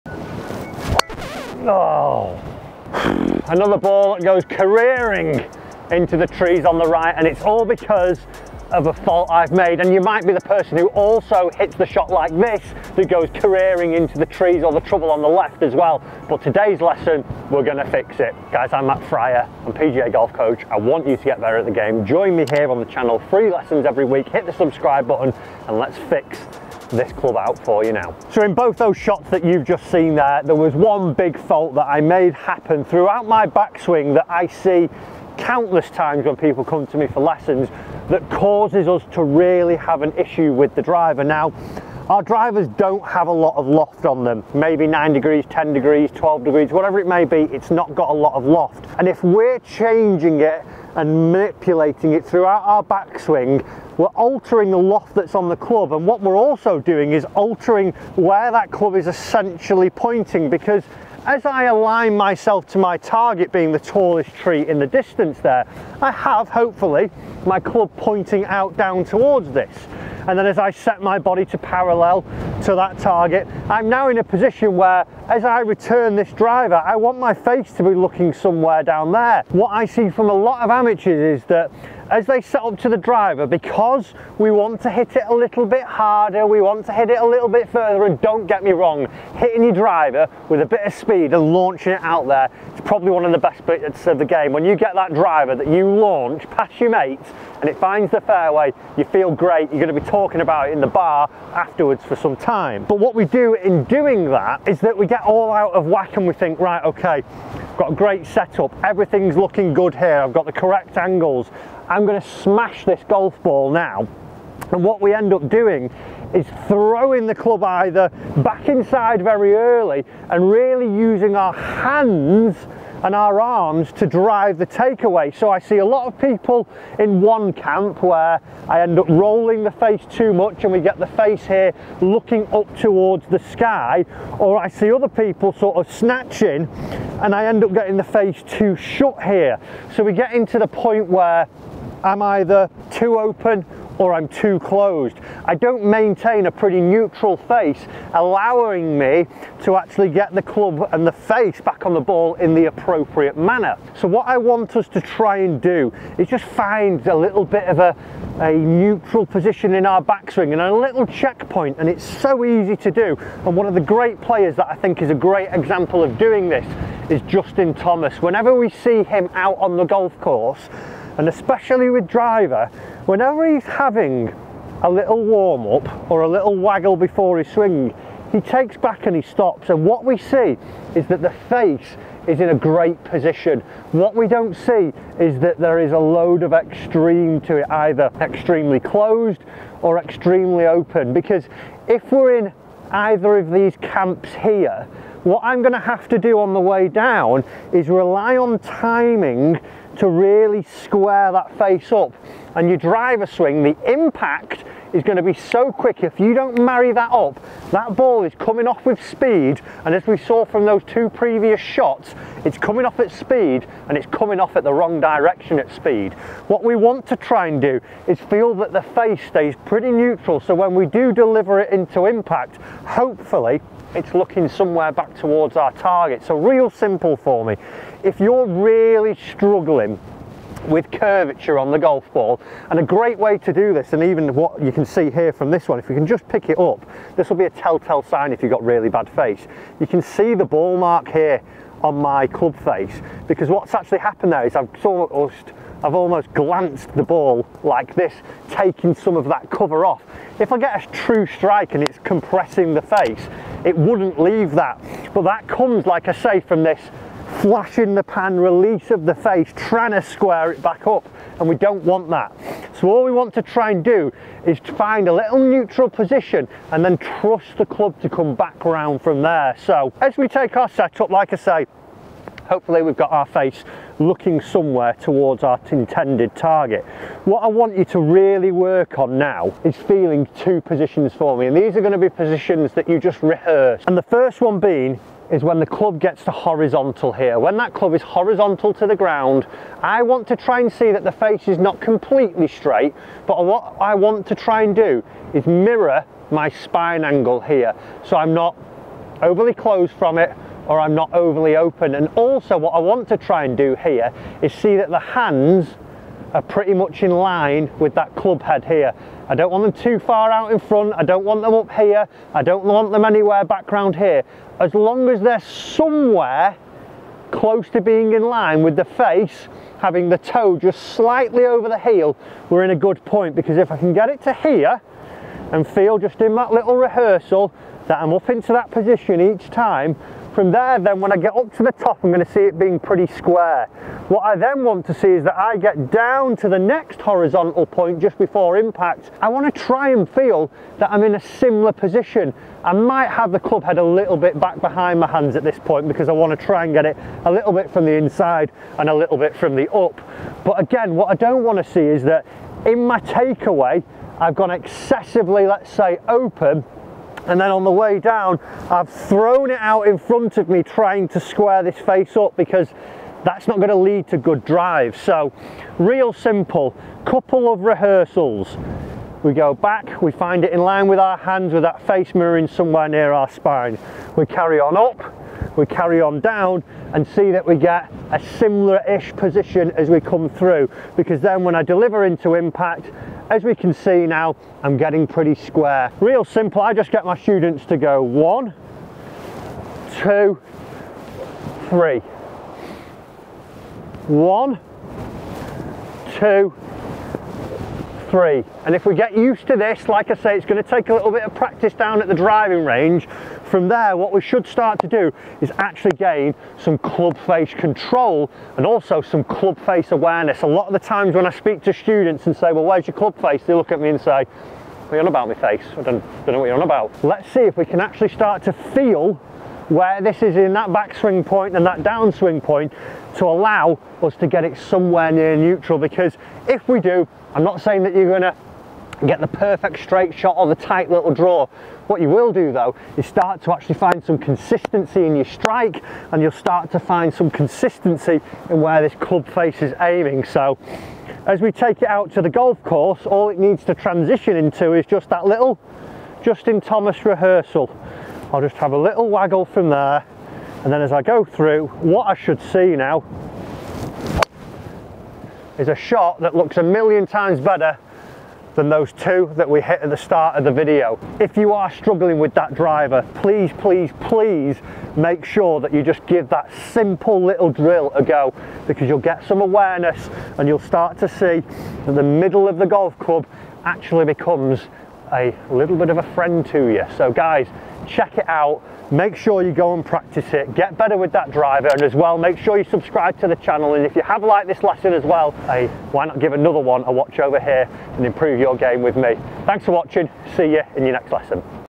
Hit A PERFECTLY STRAIGHT Drive With 1 SIMPLE Tweak! ( Golf Driver Swing Tip)
Hit The PERFECT STRAIGHT Drives With 1 SIMPLE Tweak! ( Golf Driver Swing Tip)

► Official Golf Resort Partner : The Dukes St Andrews/ Old Course Hotel